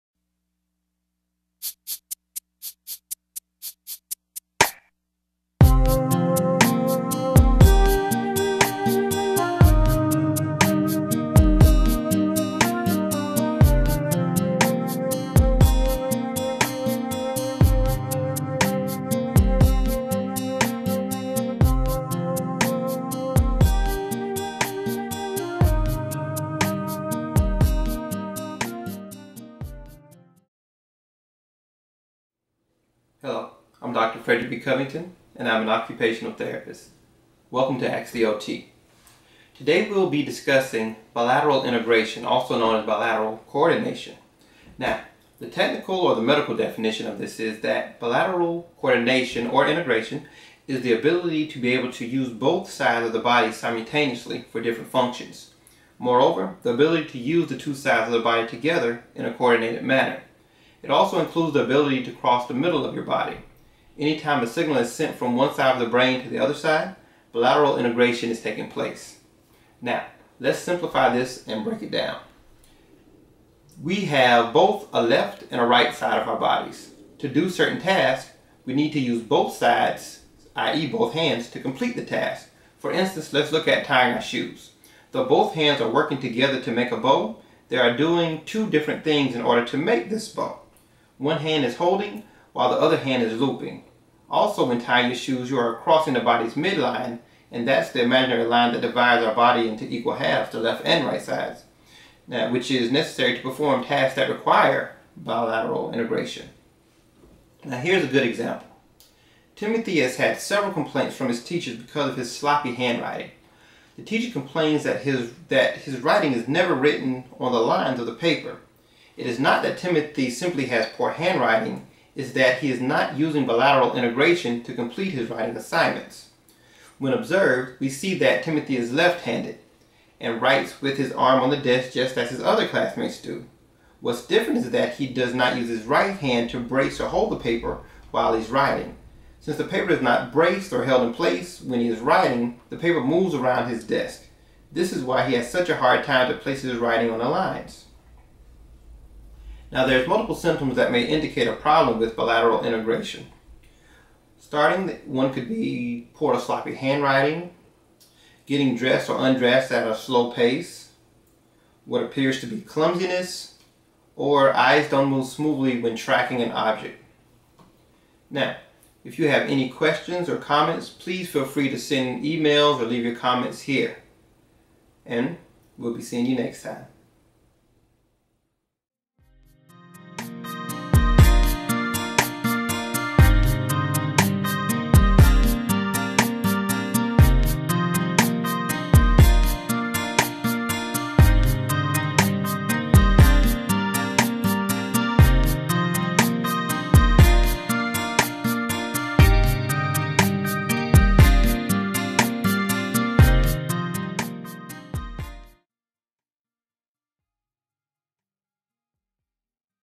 Discussing Bilateral Integration
Dr. Covington explains Bilateral Integration/Coordination via definition, simplification of medical terms, and gives examples.